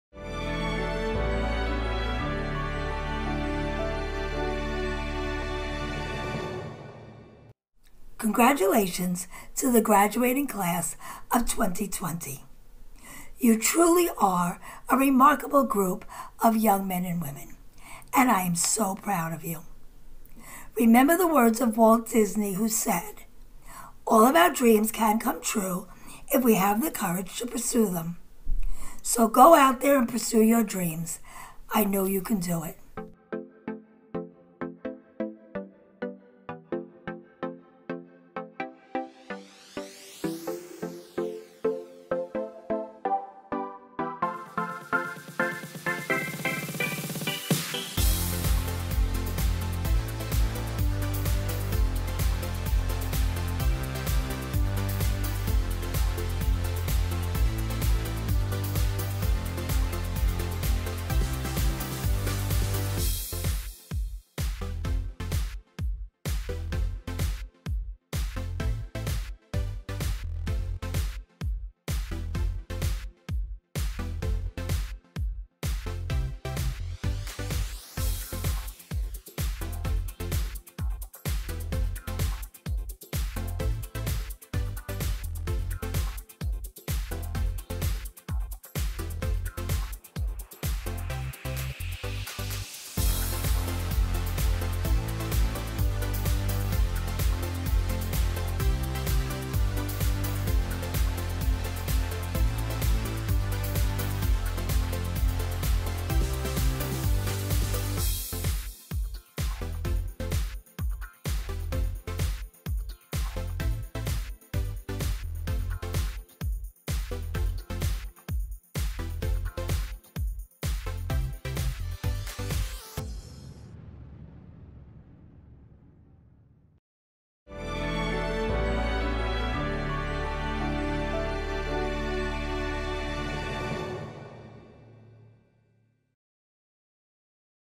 Sacred Heart Catholic Academy's Class of 2020 From NET TV Honors the Graduates of 2020
We all know that 2020 has been a challenging year but our elementary school graduating class has risen to the challenge.  Many have faced personal hardship and loss in their families and all of them have needed to adapt to remote learning and distance from their friends. This program salutes their accomplishment and ensures this milestone moment of their graduation is recognized throughout the Diocese.

Airing all summer long on NET TV!